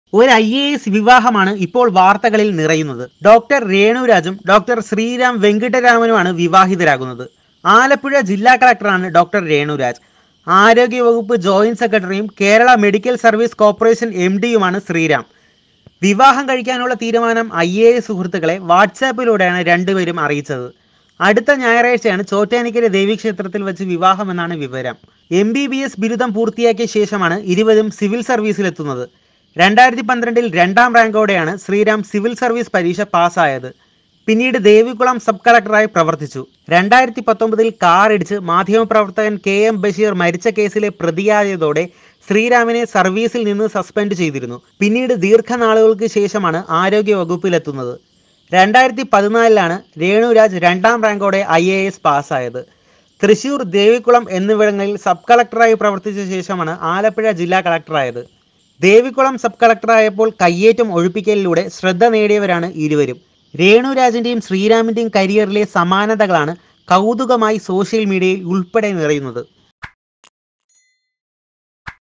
രേണുവിനും ശ്രീറാമിനും സമാനതകളേറെ! IAS officers Dr Renu Raj and Dr Sriram Venkitaraman to tie knot
This video is related to the following topics:
IAS officers
Dr Renu Raj
Dr Sriram Venkitaraman
wedding